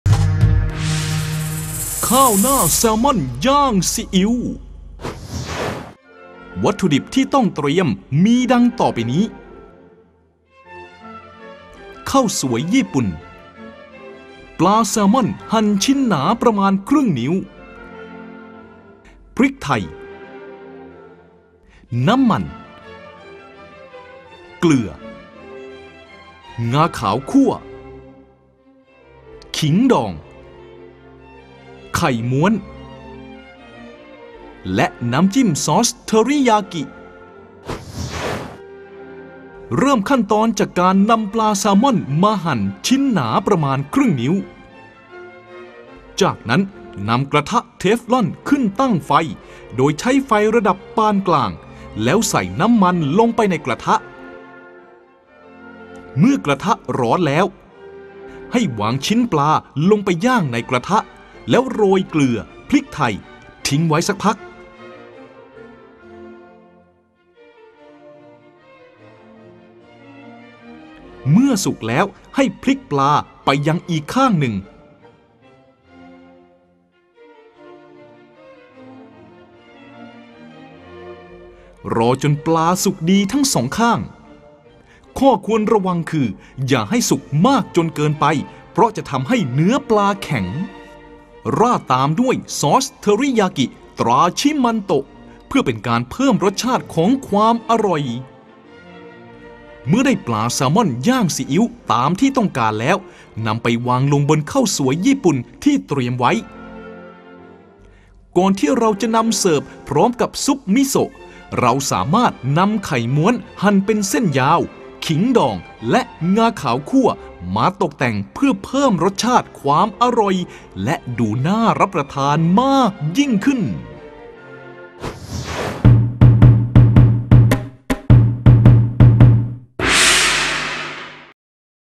FOOD PROJECT ข้าวหน้าแซลมอนย่างซีอิ้ว
เนื้อแซลมอนจะไม่คาว หวาน และนุ่มมัน โดยมีสีส้มสวย ให้คุณค่าทางโภชนาการสูงมาก ทั้งจากกรดไขมันโอเมก้า 3 , โปรตีน, วิตามิน และเกลือแร่ที่จำเป็นต่อร่างกาย
ติดตามผลิตภัณฑ์อื่นได้ที่